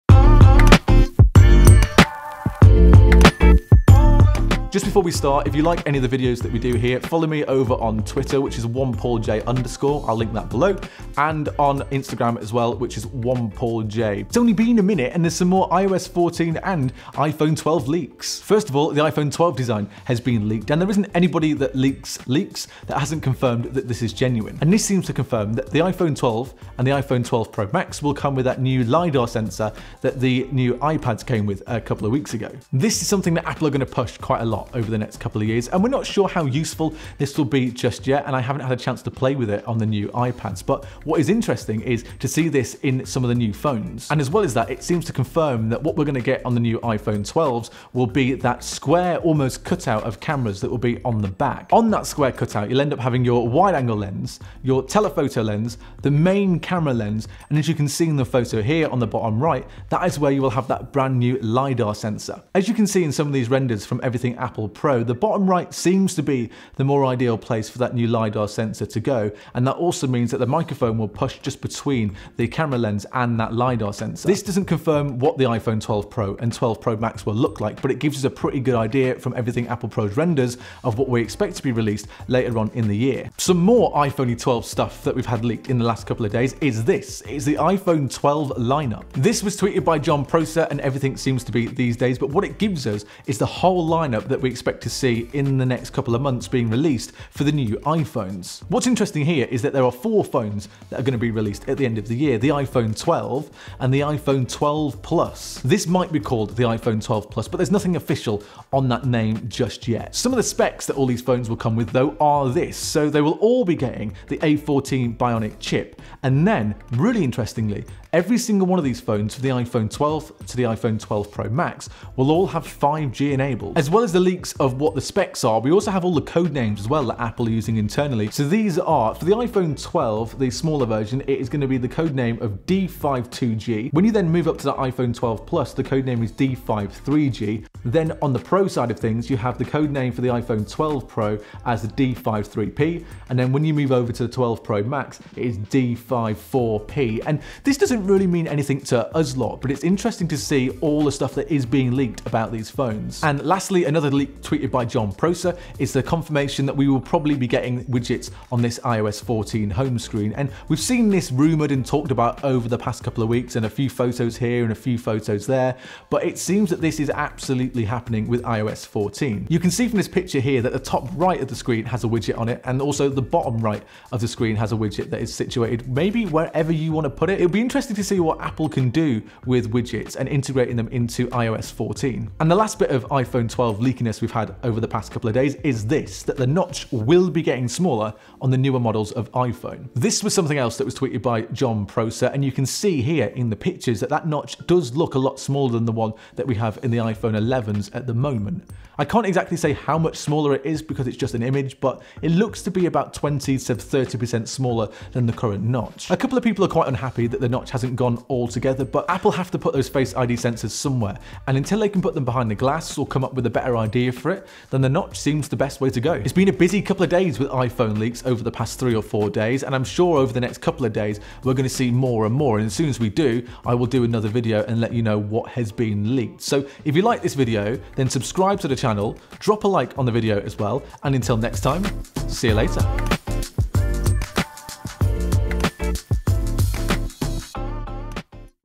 Is This The iPhone 12 Design? | IOS 14 Leaks
Some more iPhone 12 & 12 pro leaks along with some nice ios 14 stuff thrown in. Concepts in this video are from Everythingapplepro
What do you think of the upcoming iPhone 12 & 12 Pro?